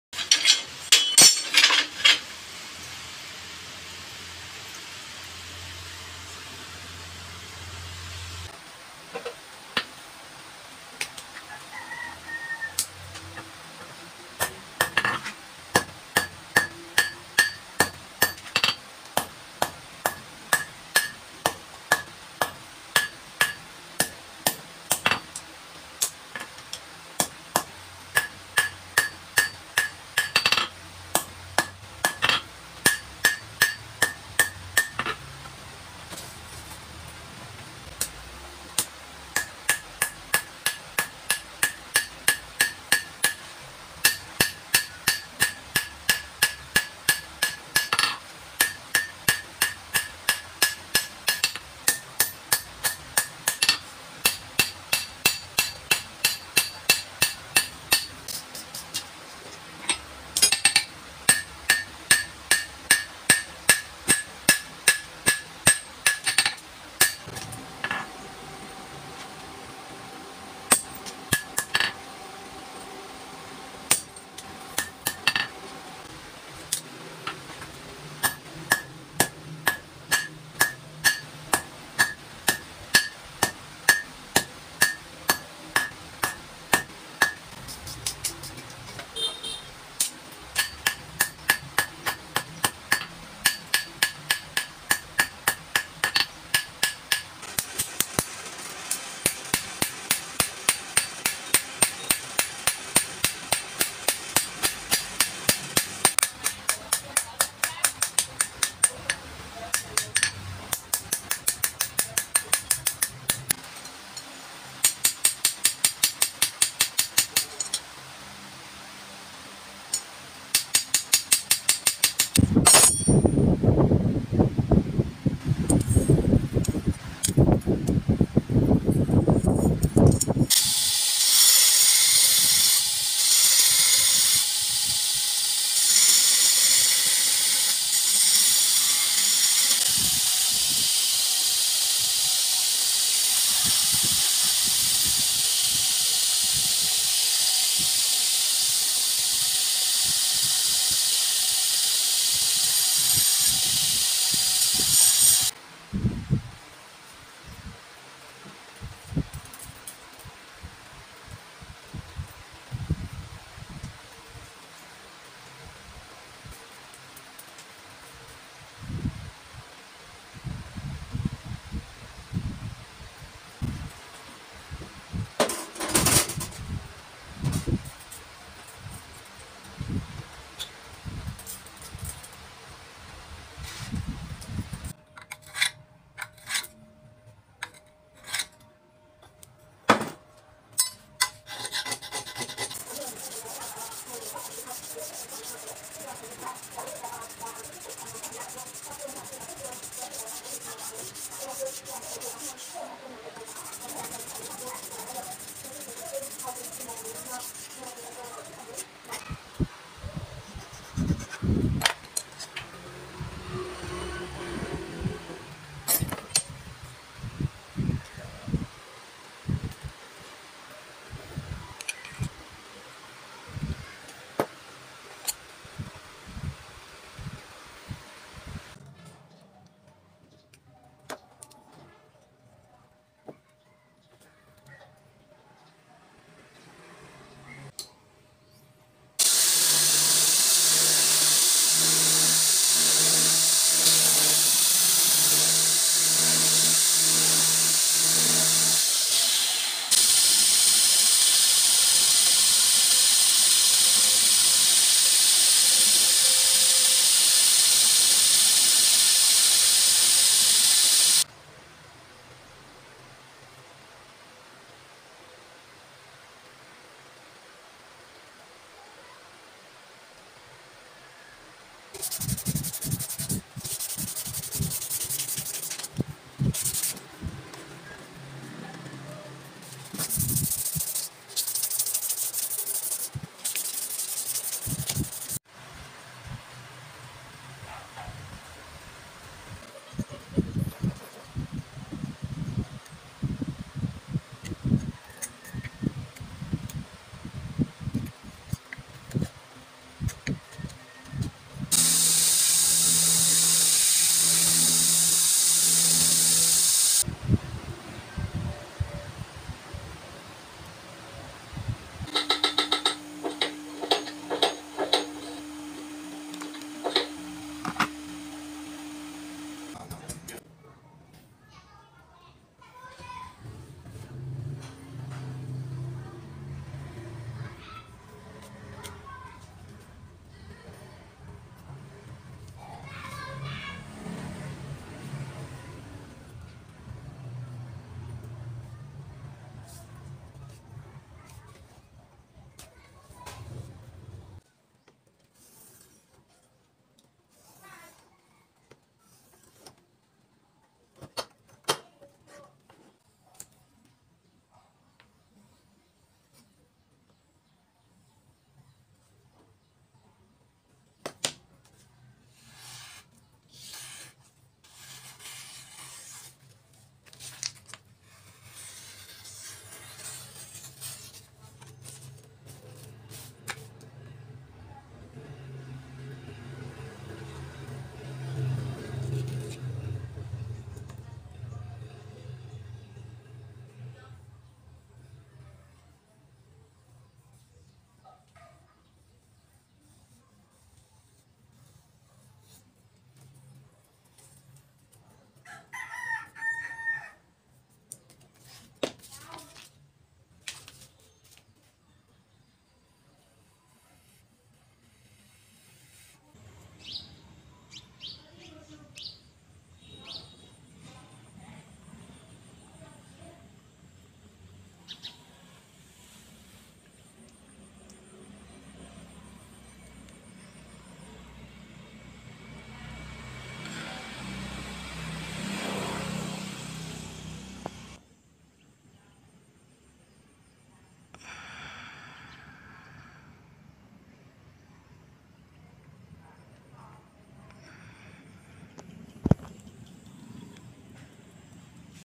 yakutia making knife.Yakut is forged from 80crv2 . high carbon steel
BTD KNIVES